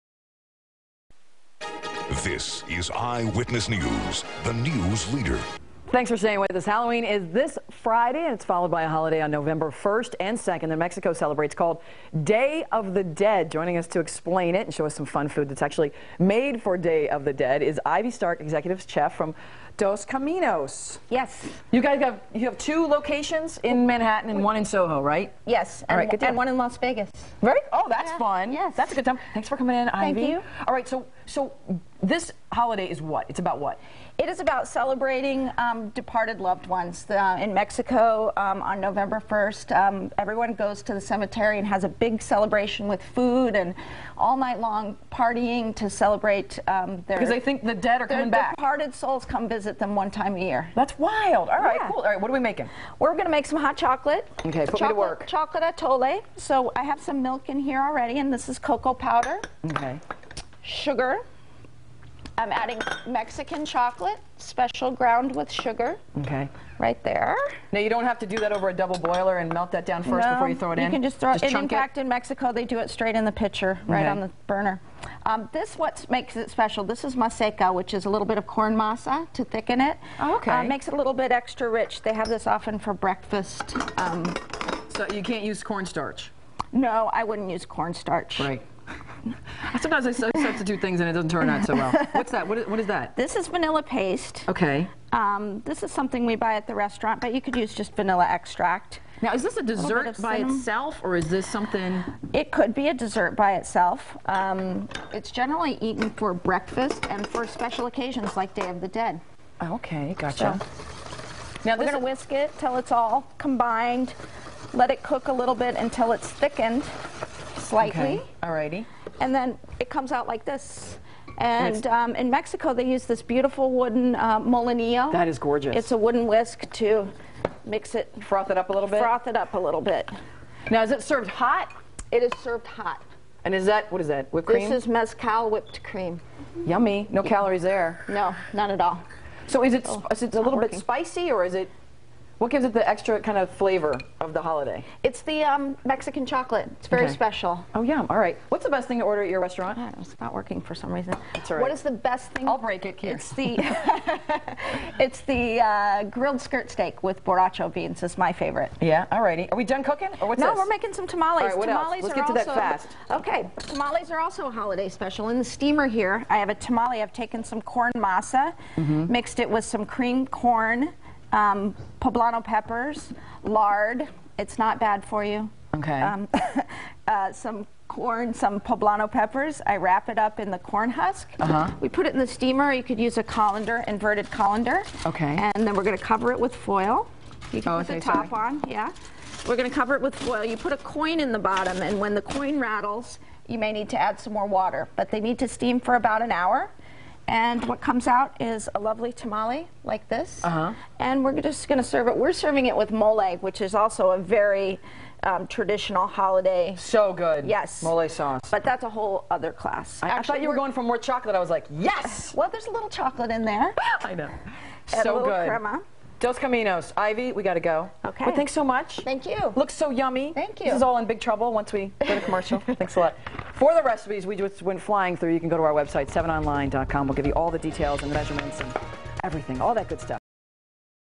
Dos Caminos- day of the dead recipes on ABC 7
Join Dos Caminos and B.R. Guest Hospitality executive chef Ivy Stark in New York City on ABC 7 for day of the dead Mexican recipes.  for more.